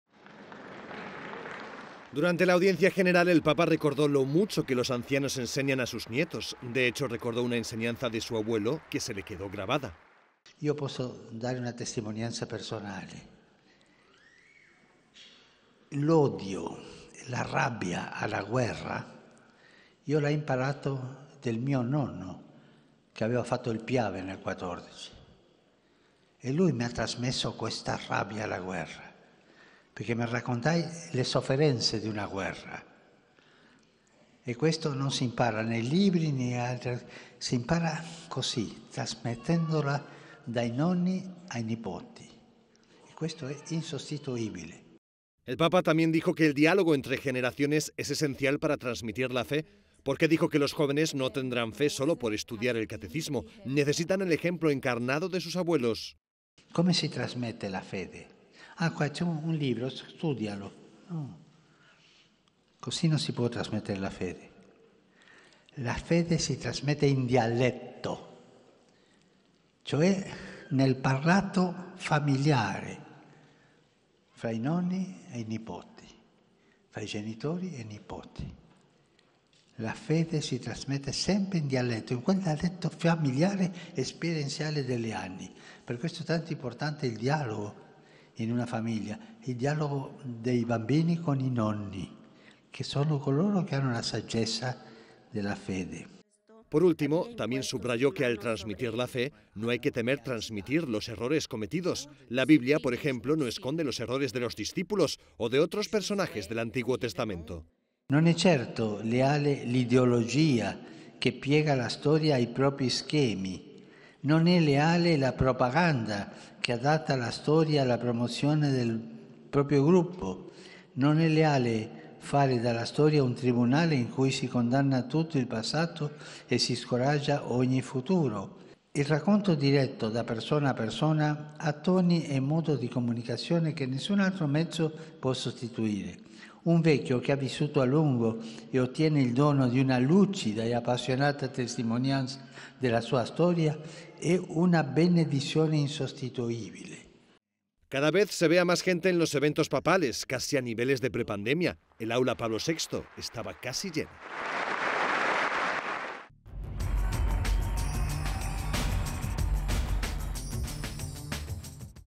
Francisco recuerda que fue su abuelo quien le inculcó “odio a la guerra”
Dijo que las enseñanzas de los abuelos son “una bendición insustituible”.